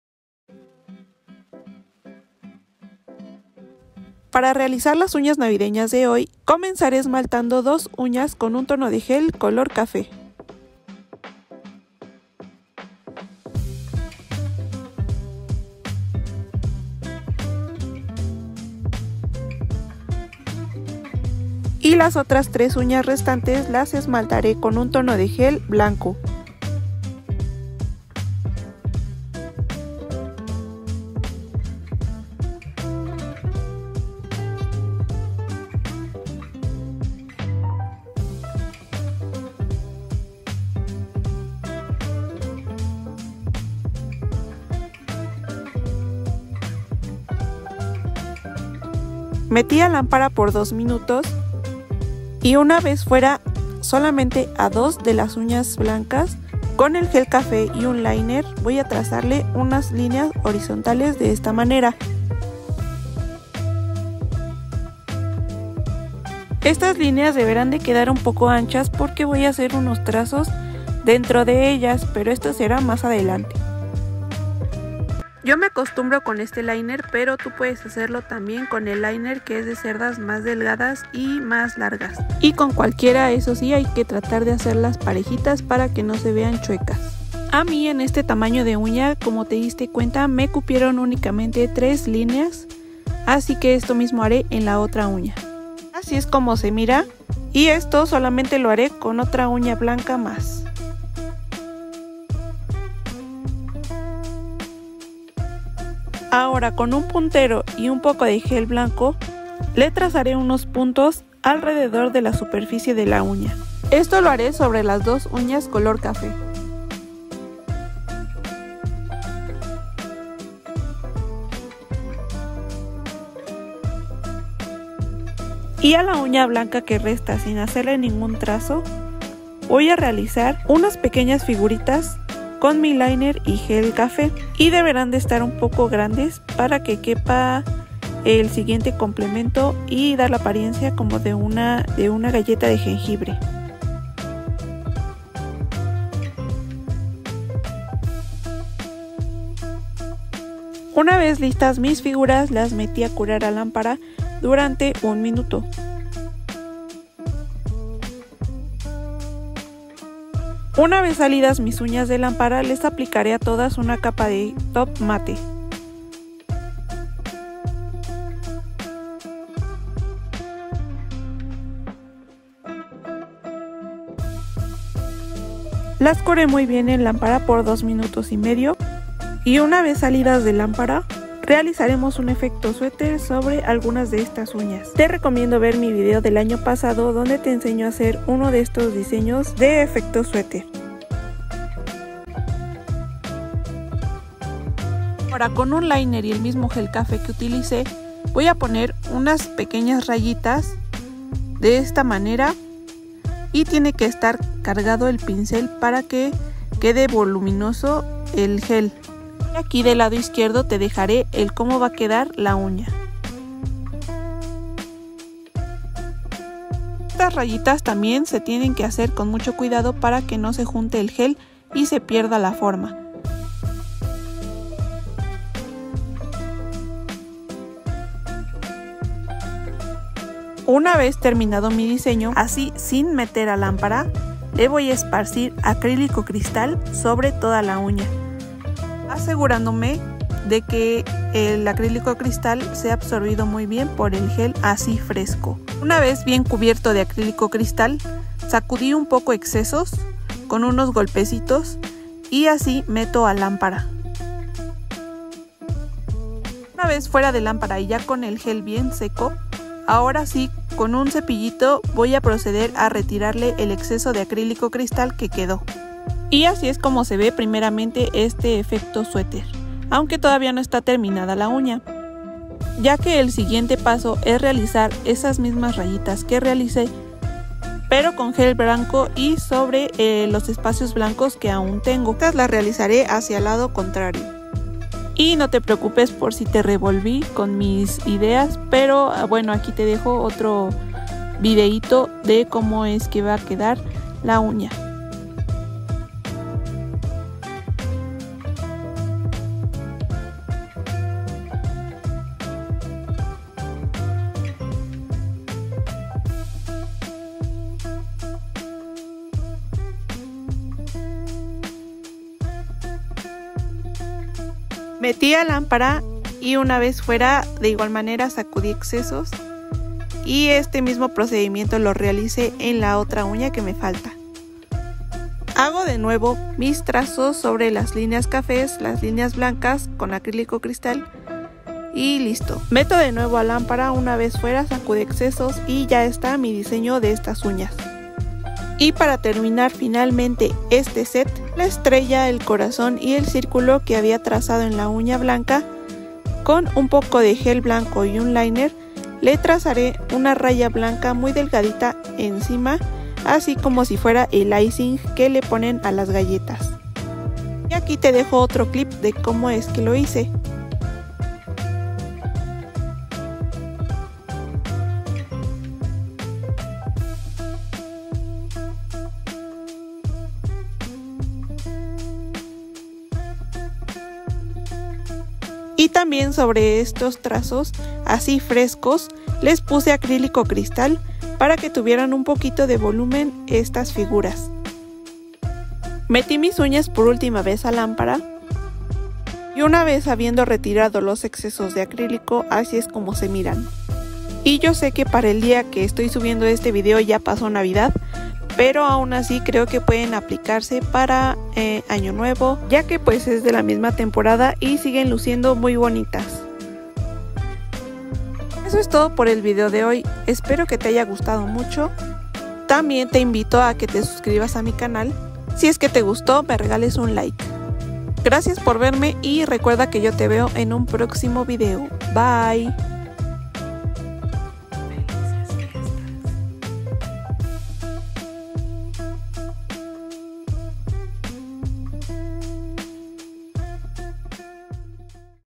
🍪💅Uñas para navidad o añonuevo ??? 😅 efecto suéter con galleta de jengibre 😍🎄🎇2️⃣0️⃣2️⃣3️⃣
Materiales utilizados en este video:
🎄     gel polish blanco
2️⃣     gel polish café/ marrón
0️⃣     acrílico cristal
2️⃣     pinceles liner
3️⃣     cepillo
🥂     top matte
🍾     lámpara uv led